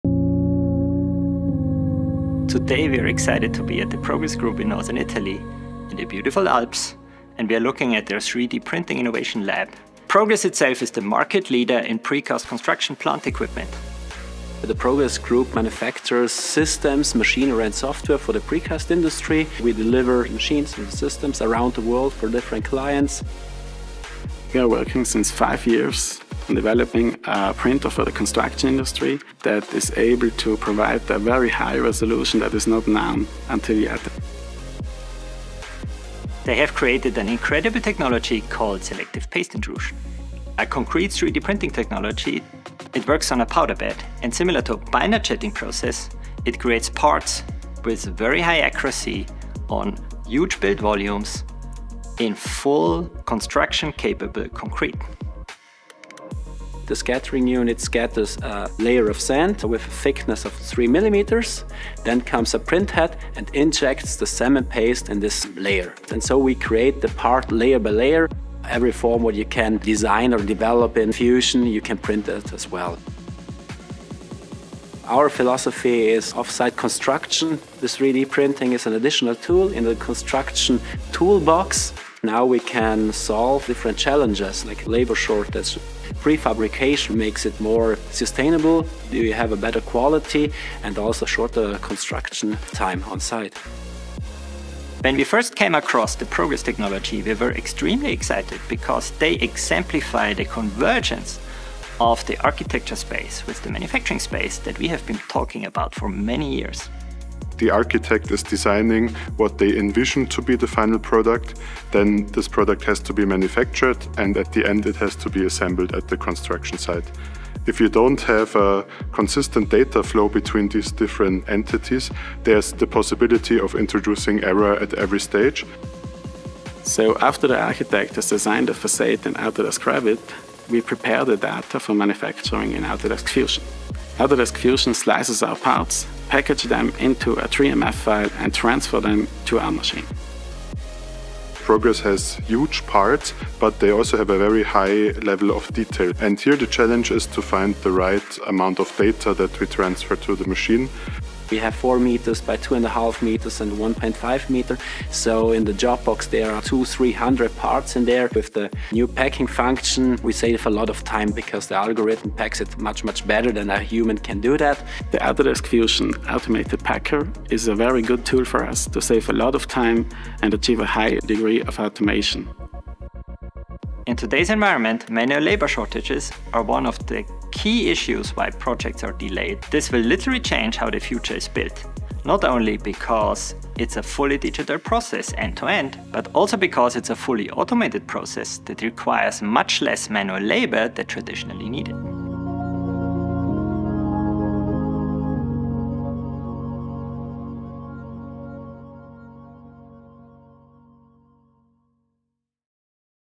How Progress Group is Automating Construction using Autodesk Fusion
The Progress Group, a global leader in precast construction plant equipment, is advancing construction automation with its innovative 3D printing technology, Selective Paste Intrusion. Developed by their 3D Innovation Lab, this high-resolution concrete printing method builds parts layer by layer, using a sand scattering unit and a precision print head. Among today’s digital construction advancements, this technology is unique; it has a solid business case and directly addresses industry challenges like labor shortages, sustainable prefabrication, and faster construction timelines.

By connecting Autodesk Revit for building design, Autodesk Inventor for detailed engineering, and Autodesk Fusion for manufacturing, Progress Group showcases how Autodesk’s product converges the Manufacturing and Construction industries.